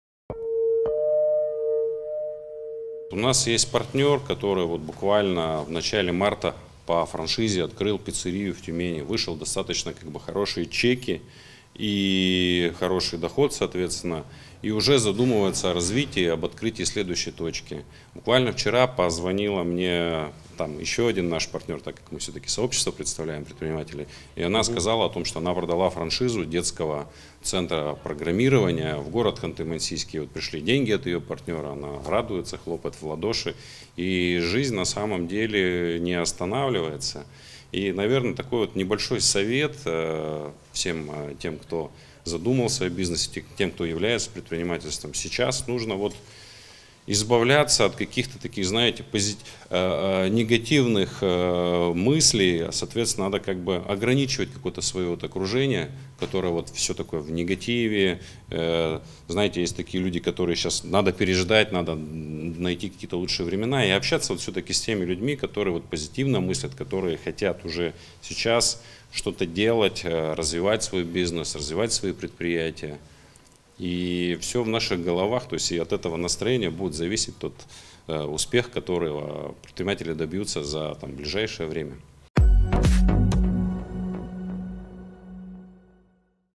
Нужно избавляться от негативных мыслей
Несмотря на очевидные сложности, участники рынка франчайзинга продолжают развивать проекты.

«У нас есть партнер, который в начале марта открыл по франшизе пиццерию. Успел выйти на достаточно хороший средний чек и доход. Он задумывается уже об открытии следующей точки. На днях еще один партнер продала франшизу детского центра по программированию в город Ханты-Мансийск. По своему бизнесу не скажу, что ничего не поменялось. Несколько предпринимателей, с которыми работаем, поставили бизнес на паузу. Другие видят возможности - выходят на новые проекты и регионы. Значит будем работать», - привел примеры региональный представитель Российской ассоциации франчайзинга в УФО.